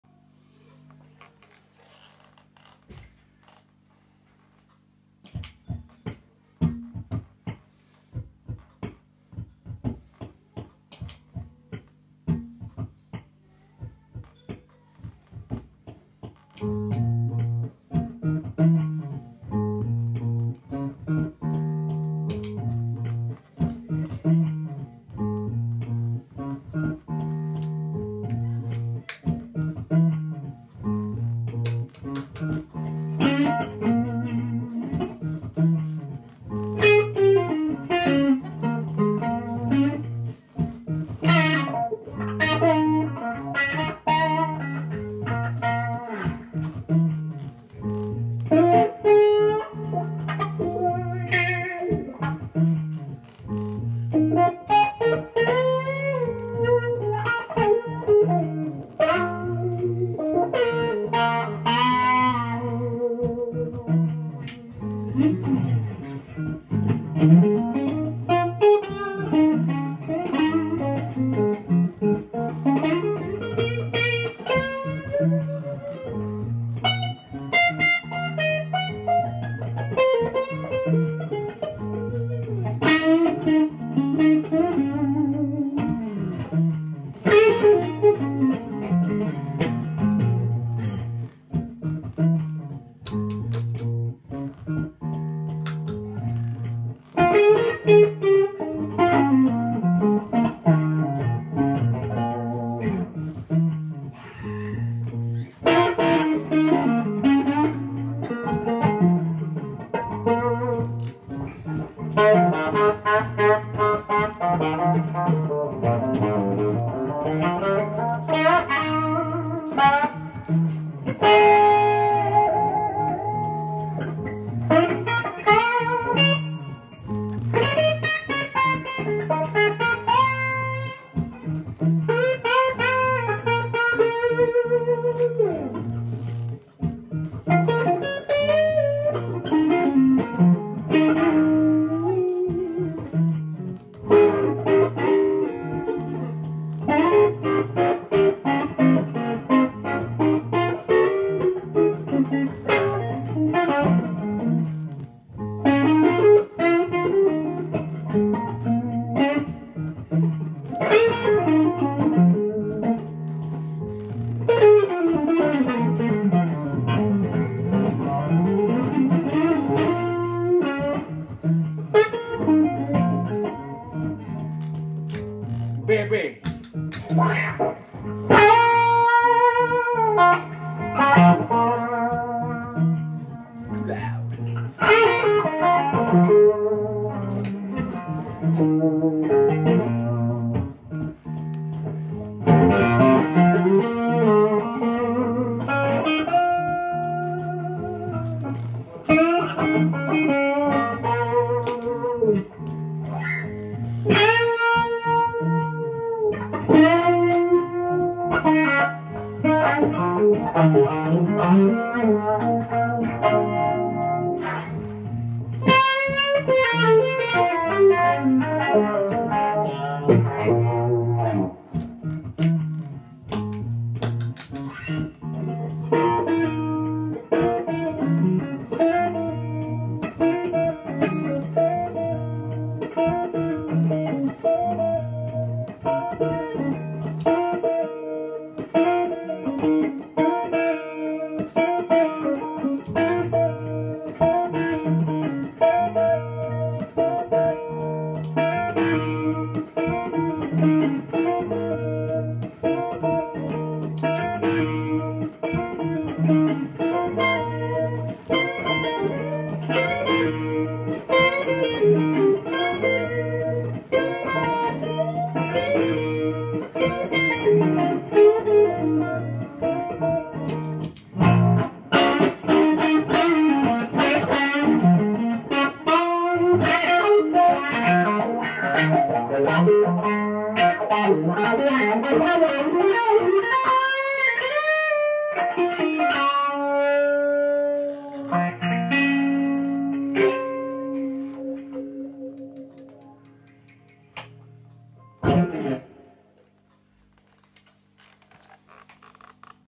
Ditto looper jam. Trying different drive sounds - amp only them Jetter GS124 then BB Preamp
Apologies for poor camera work! -
Strat with Duncan TB4 bucker & Fralin Vintage hots into a Ceriatone OTS Bluesmaster - comparing amp drive with Jetter GS124 & Xotic BB drive pedals / 'drum beat' is just me slapping the damped strings at the start & looped with a mini bass line with the brilliant TC Ditto

Ceriatone Bluesmaster Amp drive at start - Jetter at 1:35, BB Preamp at 2:55